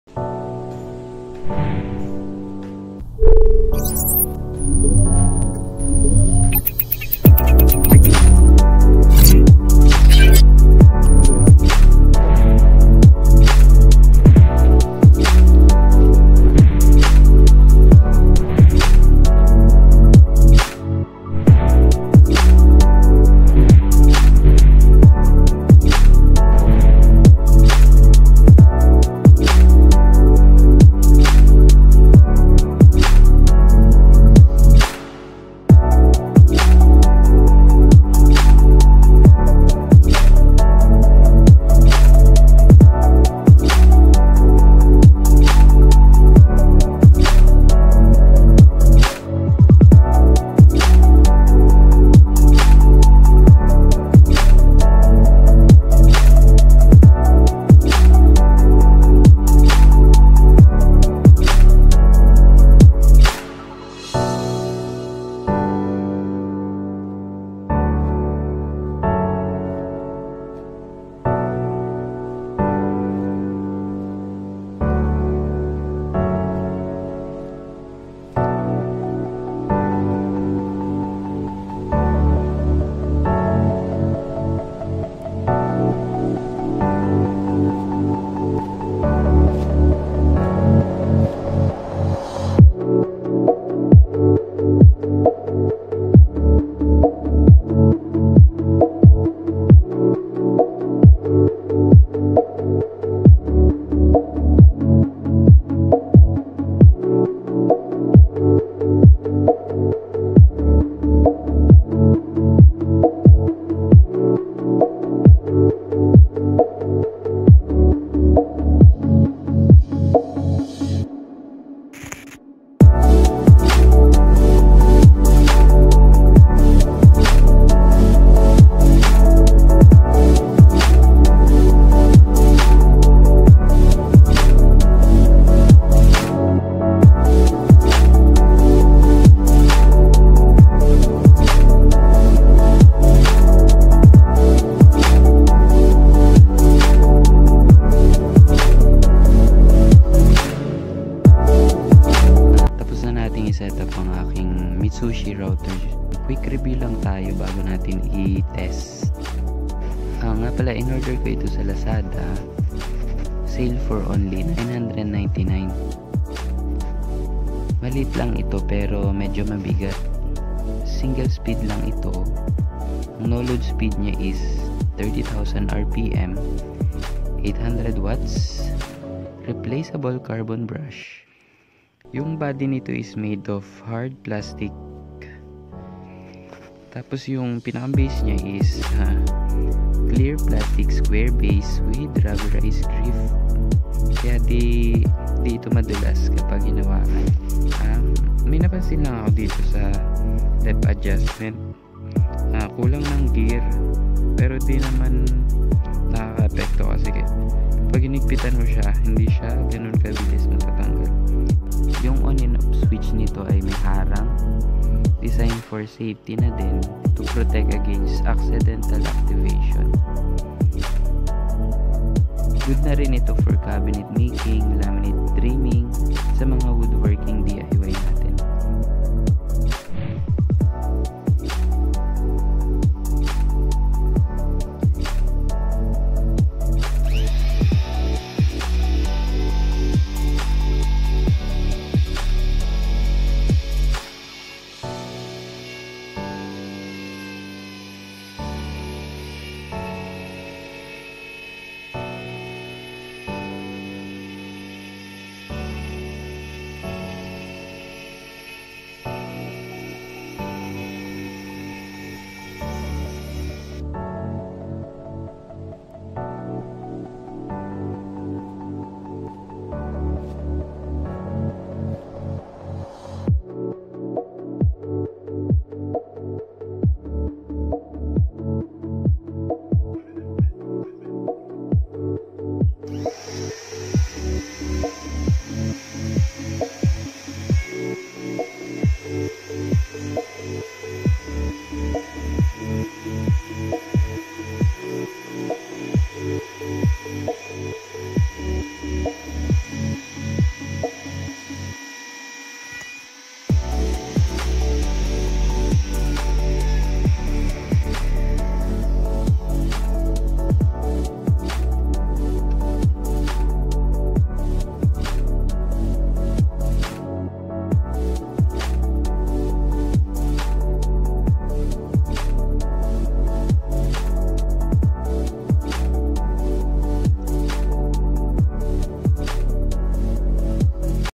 MITSUSHI TRIM ROUTER
Trim routers are very handy tool to have because they're so easy to handle with one hand.

Mitsushi elictric trimmer
Rated power: 800w
Rated Voltage: 220v
Rated frequency:60Hz
No load speed:30000 RPM
Capability: 6mm 1/4